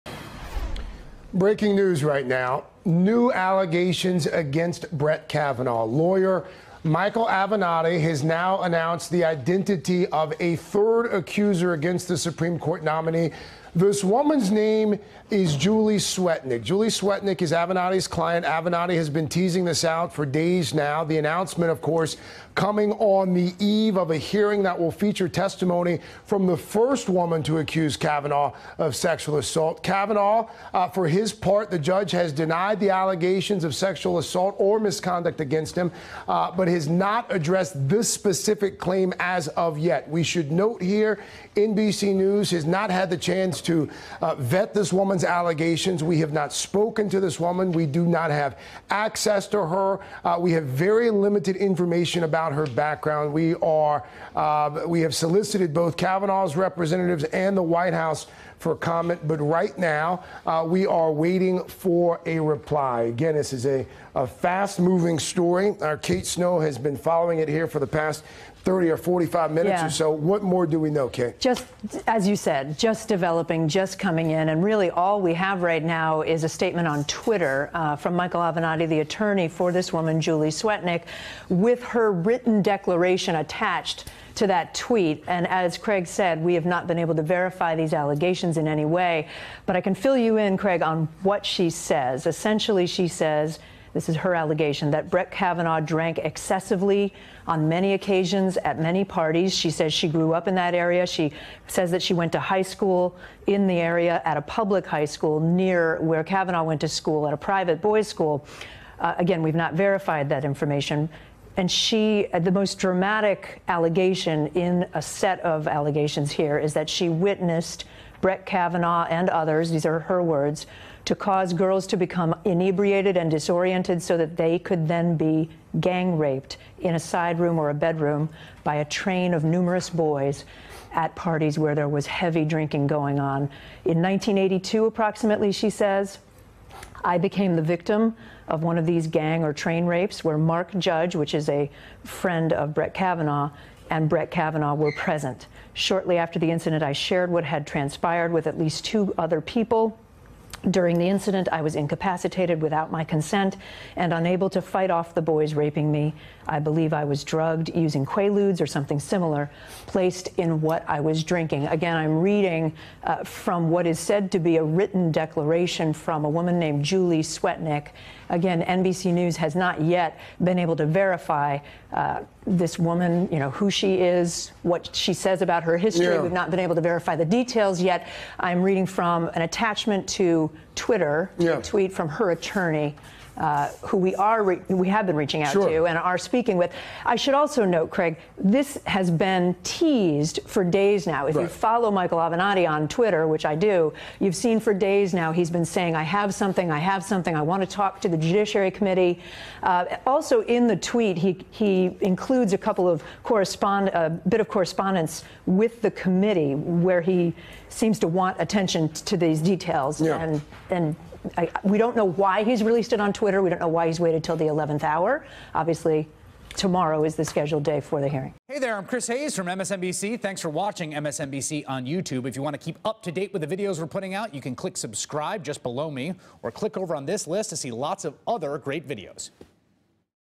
Third Accuser Comes Forward With Allegations Against Brett Kavanaugh | Craig Melvin | MSNBC
A third accuser comes forward with allegations against Supreme Court nominee Brett Kavanaugh saying he and his friend, Mark Judge, were present at a 'gang rape.'